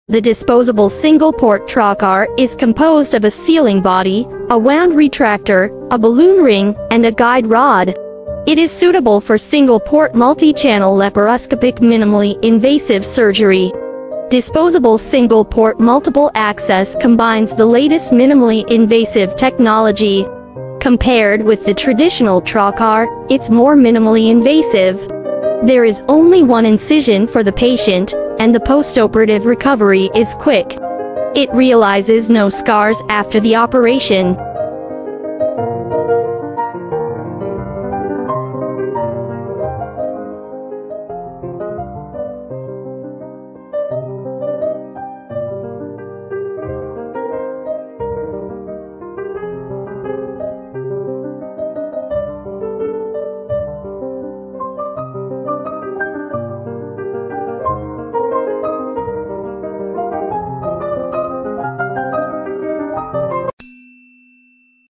multiple access disposable single-port trocar
The disposable single port multiple access from our reliable manufacturer and supplier, also called disposable single-port trocar, is used for specialists to establish a access for multiple surgical instruments to enter and leave the abdominal cavity from the outside in one hole, and deliver gas to the abdominal cavity. Compared with the traditional trocar, this product designed by our factory in China is more minimally invasive.
Zhejiang Geyi Medical Instrument Co., Ltd.
Factory Address: No. 5, Hutang Road, Xiaya Town, Jiande City, Hangzhou, Zhejiang Province, China
Headquarters: A-701, No. 333, Jianghong Road, Changjiang Street, Binjiang Zone, Hangzhou, Zhejiang Province, China